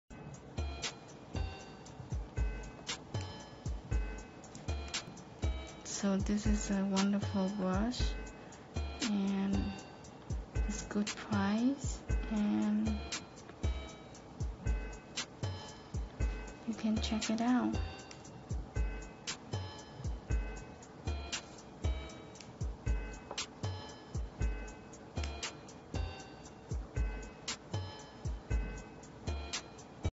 100% Pure Kolinsky Sable Hair Red Wooden Handle Nail Art Brush
The 100% Pure Kolinsky Sable Hair Red Wooden Handle Nail Art Brush made of Kolinsky hair are widely known to be the finest available, offering spectacular artificial nail-fashioning results. A "Kolinsky" is a Siberian member of the weasel family with tail hairs which are perfect for brushes for professional nail-sculpting. Only the finest materials, master crafted by experts A great amount of work goes into the creation of each and every brush Fine brush-making is a true art Acrylic handle, acetone-resistant.